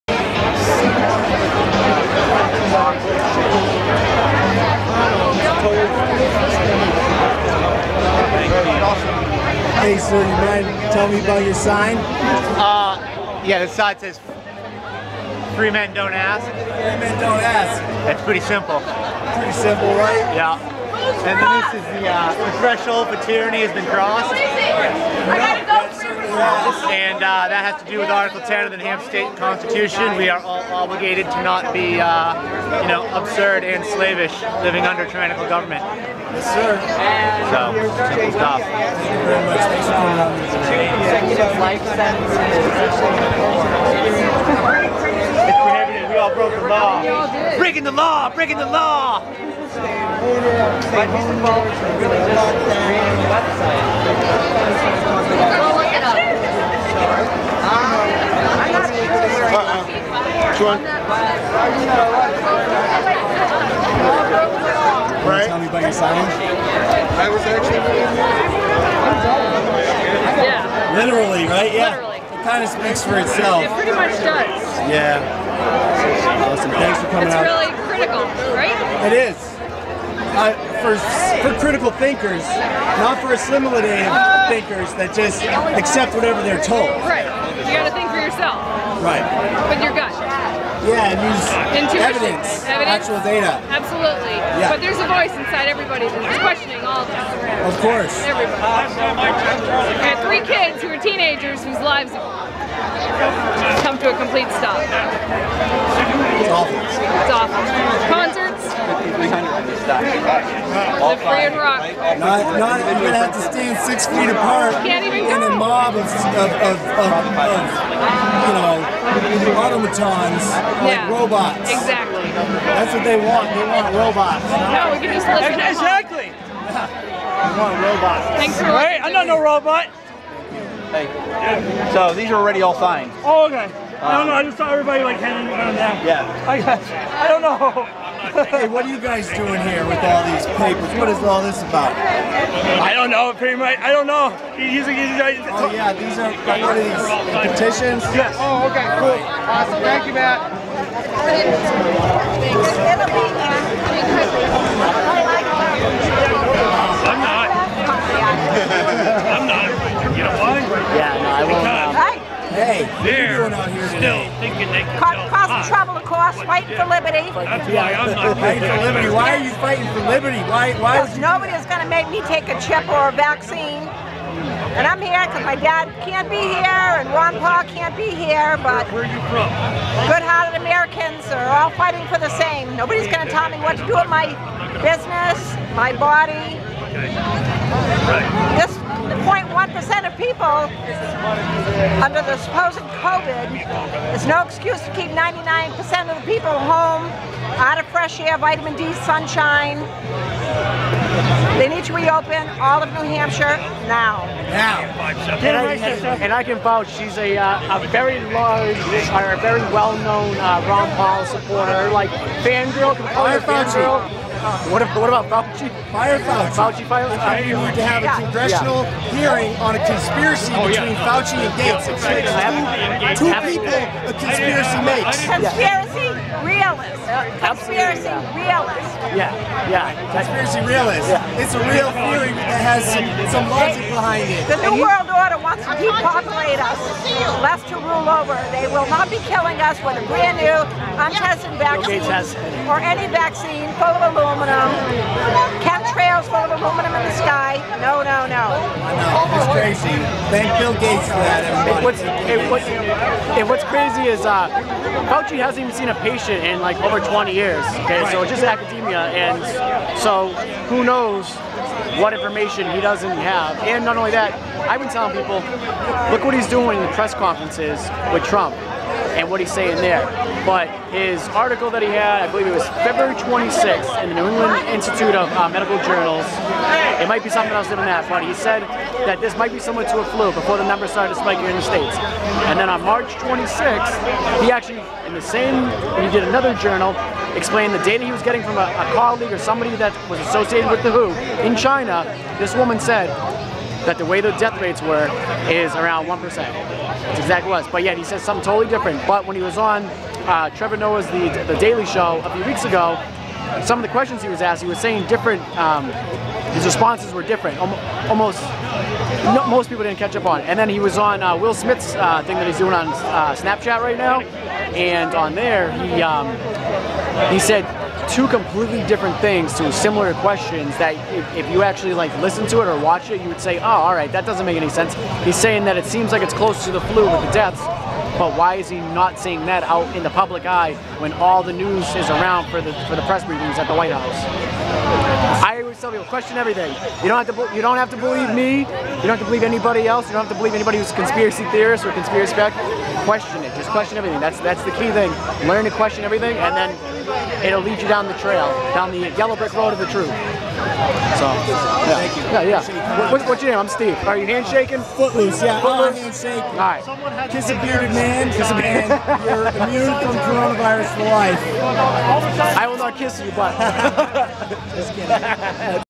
Free Men Don't Ask
We don't need permission! Right after the group photo on the State House steps at the 5/2/2020 Rally to EndTheLockdown and OpenNH!